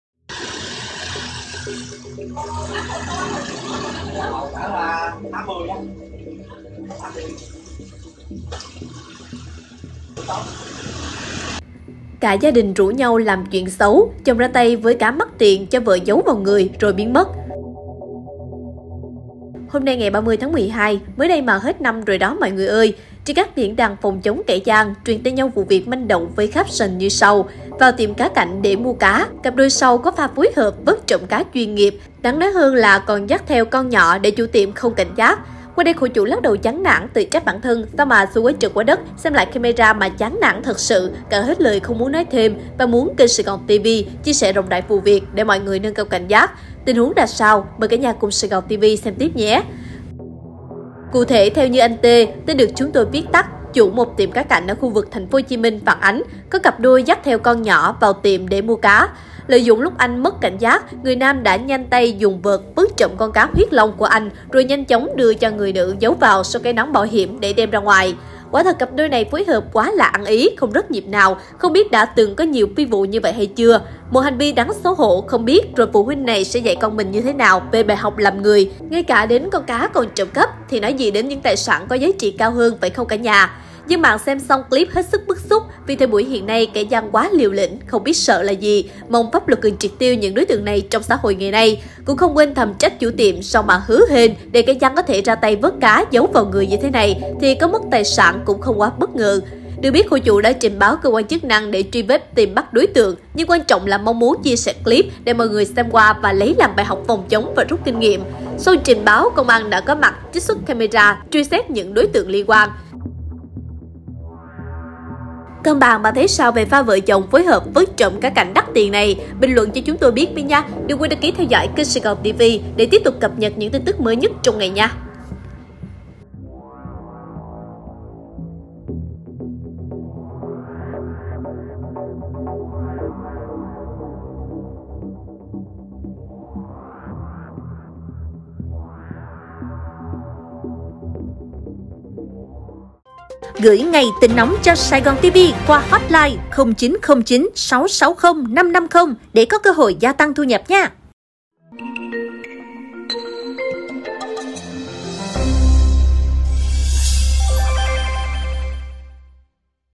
Cả gia đình RỦ NHAU LÀM CHUYỆN XẤU… chồng ra tay VỚT CÁ “MẮC TIỀN” cho vợ GIẤU VÀO NGƯỜI | SaigonTV
Cụ thể, Theo như anh T. chủ một tiệm cá cảnh ở khu vực TP.HCM phản ánh có cặp đôi vào tiệm để mua cá. Lợi dụng lúc anh mất cảnh giác người nam đã nhanh tay dùng vợt vớt trộm con cá "huyết long" của anh rồi nhanh chóng đưa cho người nữ giấu vào sau cái nón bảo hiểm để đem ra ngoài.

| Nguồn MXH